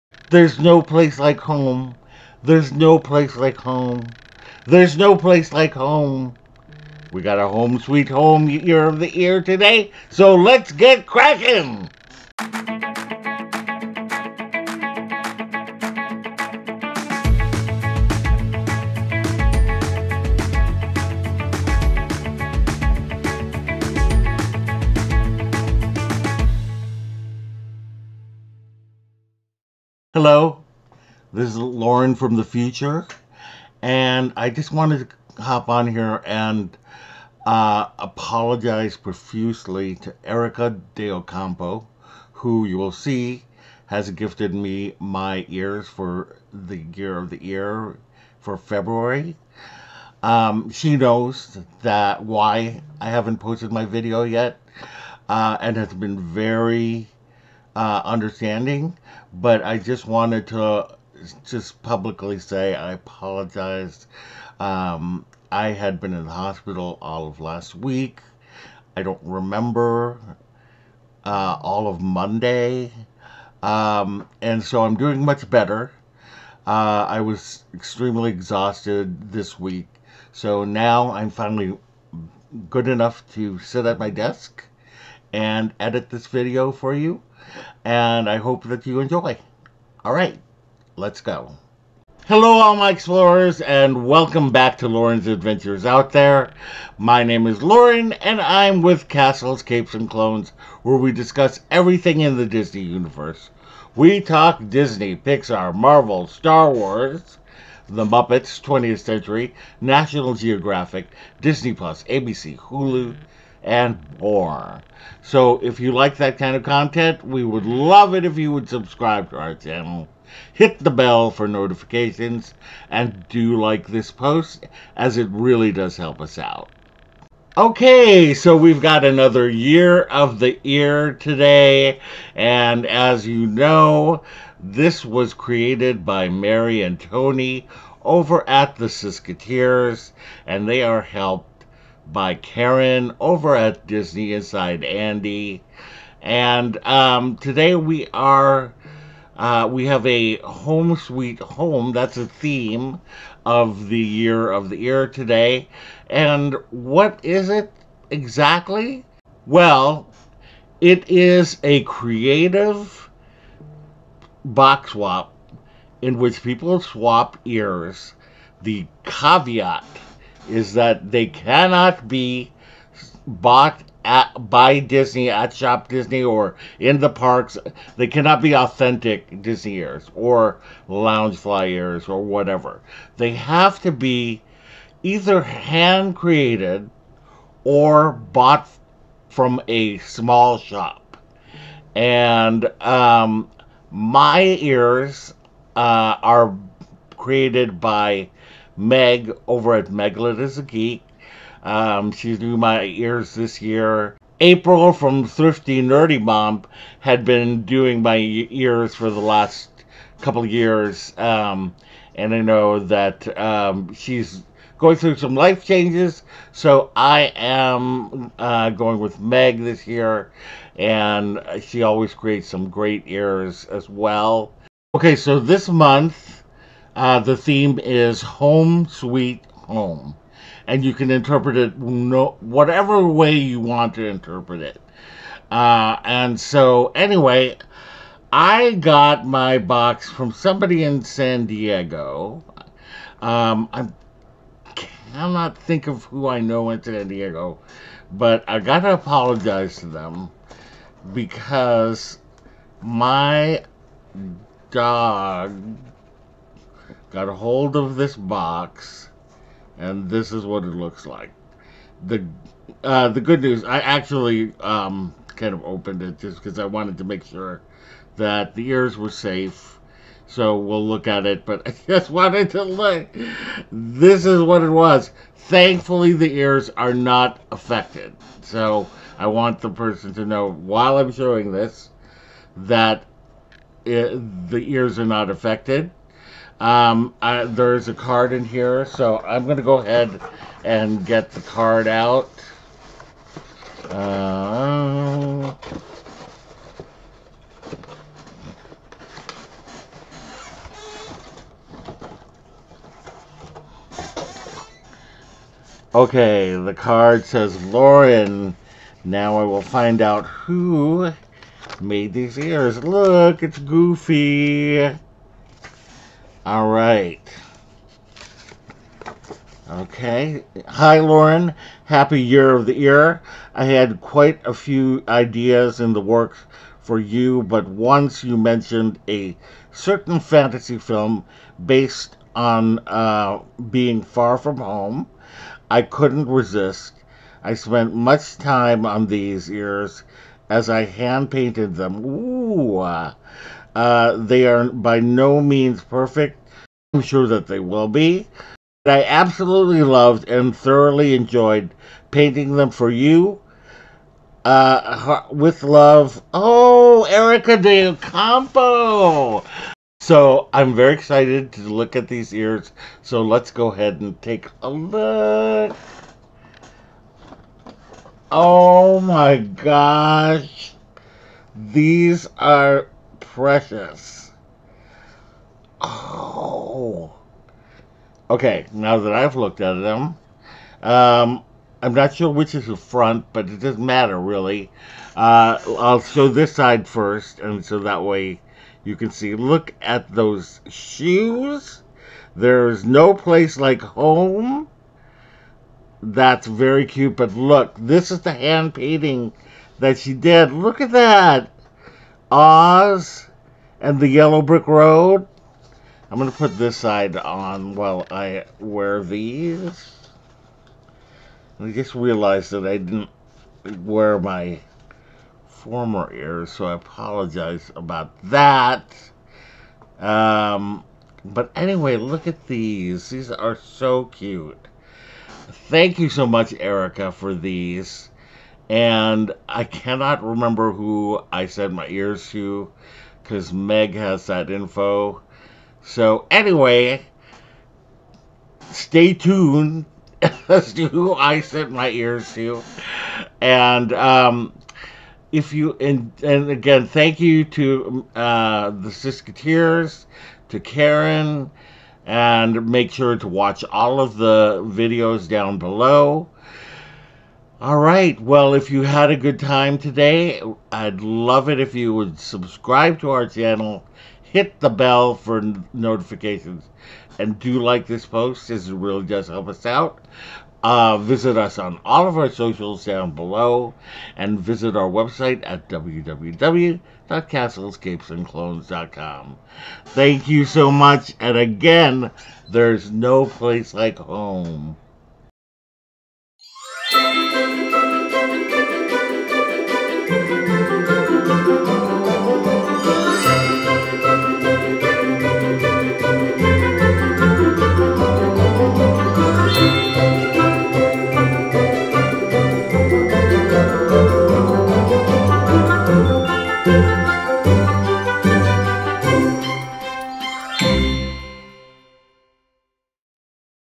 YEAR OF THE EAR | February 2024 | HOME SWEET HOME
In this episode, Castles, Capes & Clones Co-Host Loren participates in Year of the Ear hosted by the SistketEars and Karen from Disney Inside Andy. This month's theme is "Home Sweet Home."
Creators

Participating Channels